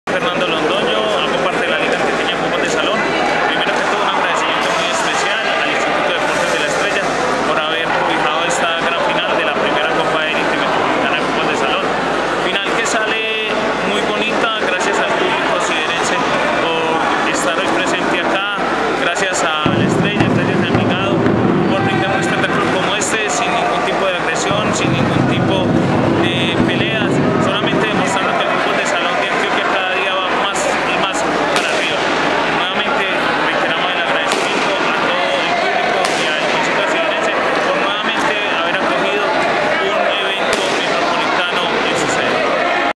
Palabras Luis Fernando Londoño
Coordinador de La Liga Antioqueña de Fútbol de Salón. Felicita a La Estrella y al público Siderense.